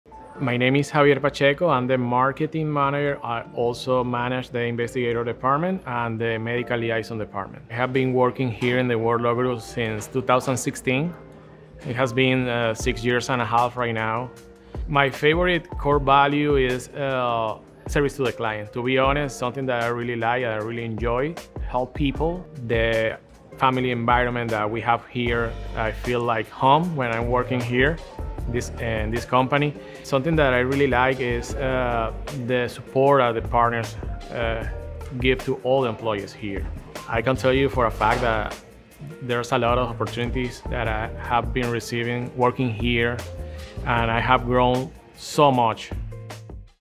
Service to the client | Testimonio Javier P. | The Ward Law Group
Acabamos de celebrar nuestro décimo aniversario, y no estaríamos aquí sin todo el equipo humano que trabaja día a día para que este sueño, que ahora es una realidad, siga creciendo.
Por eso, hoy nos enorgullece destacar a nuestro Marketing Manager ⭐ Javier Pacheco!⭐

Estamos muy felices de que Javier forme parte de la familia WardLaw desde hace 6.5 años! Somos afortunados de tener su talento y su dedicación, siempre dispuesto a servir ¡Miren su testimonio aquí!

We just celebrated our tenth anniversary! We would not be where we are without the team that works every day.
For this reason, today we are proud to highlight our Marketing Manager ⭐ Javier Pacheco!⭐

We are very happy that Javier has been part of the WardLaw family for 6.5 years! We are fortunate to have his talent and dedication, he is always ready to serve! Check out his testimony here.